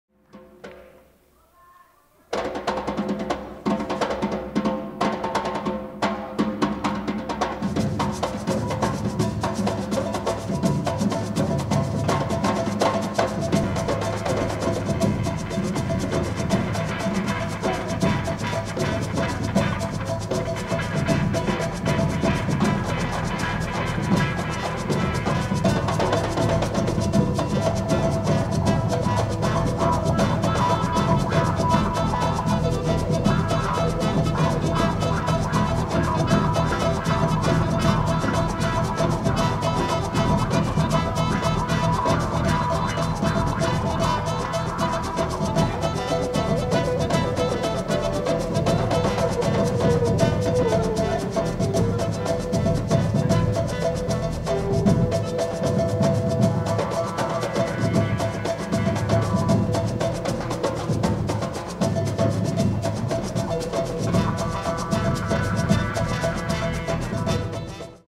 Axe Capoeira Victoria 10th Anniversary
These are scenes from the Axé Capoeira Victoria 10th Anniversary DVD, March 11-14th 2010.